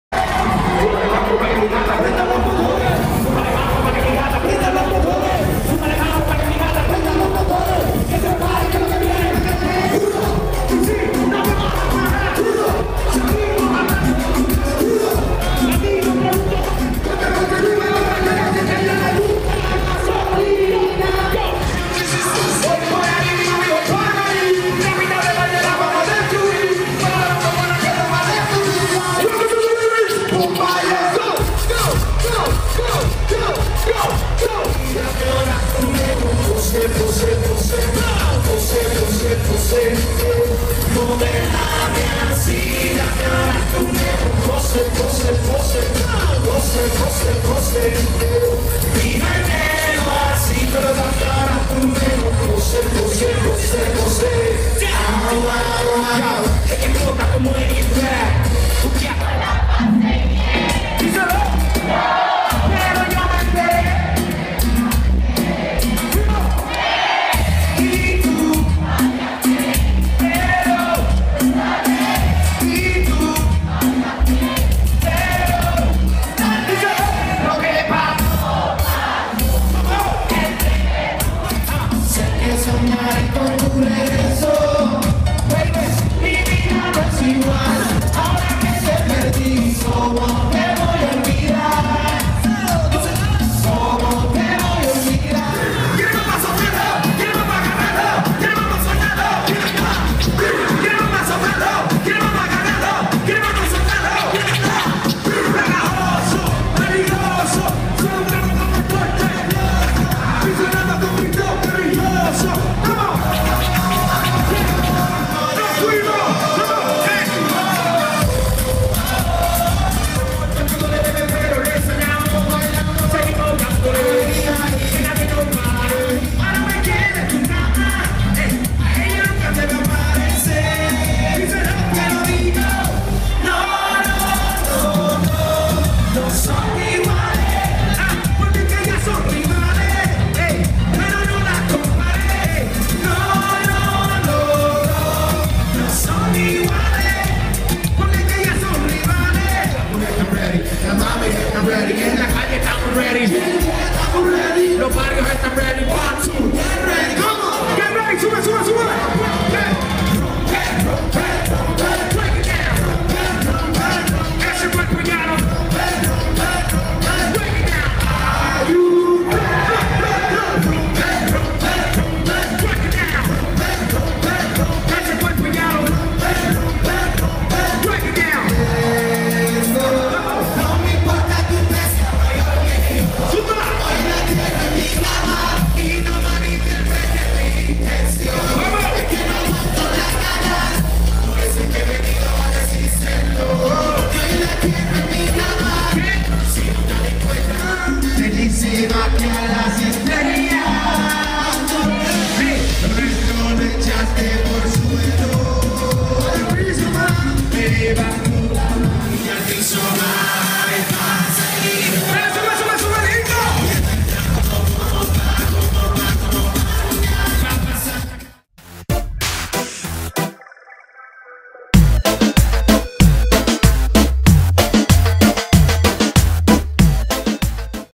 Recap: Daddy Yankee @ Echostage (2014)
Daddy Yankee performing at Echostage in Washington DC on July 18th, 2014.